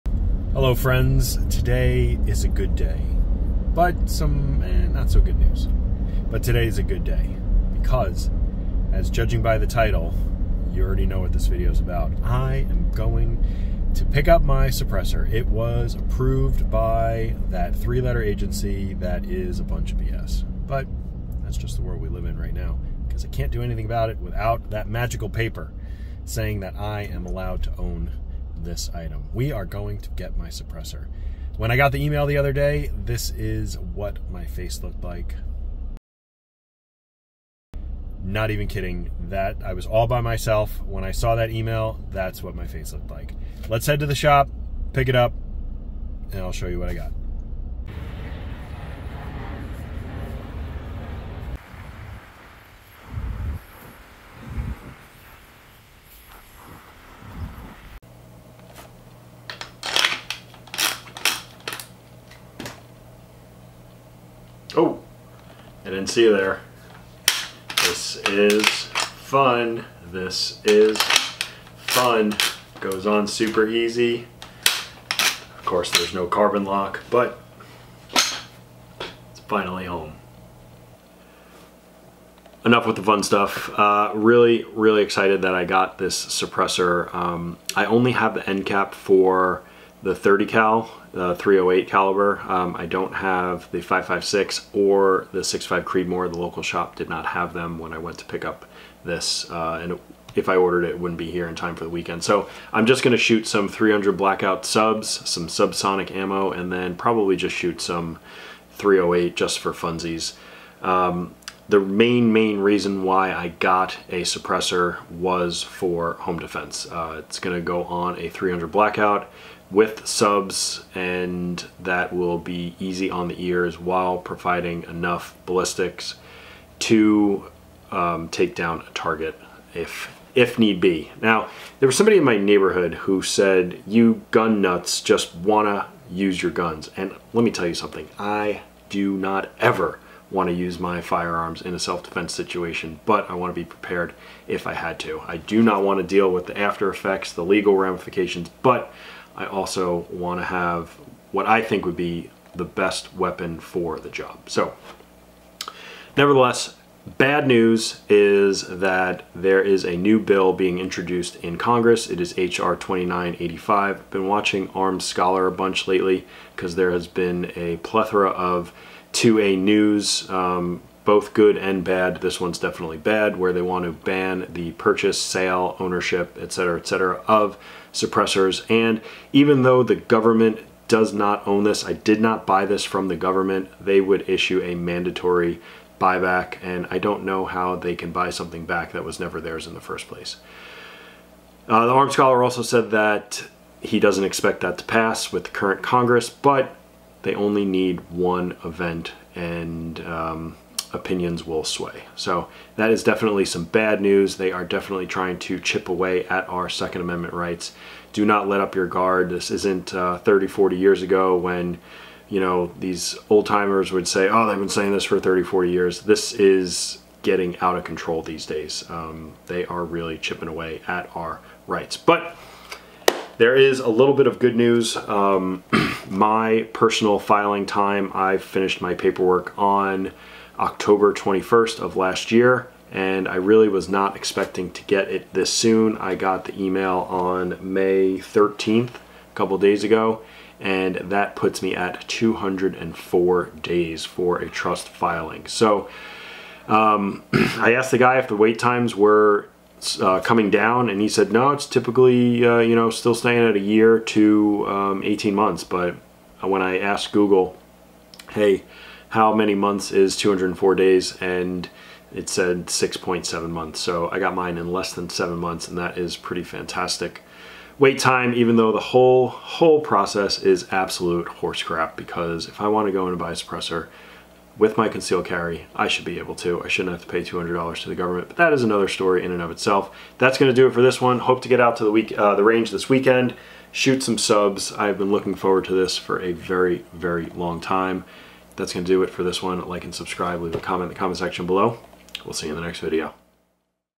Got my First Suppressor ... Just in time for it to be BANNED! E-File time UPDATE!
Can't wait to get out there and shoot 300BO subs thru the Sandman S without ear protection. Something I've looked forward to for a LONG time.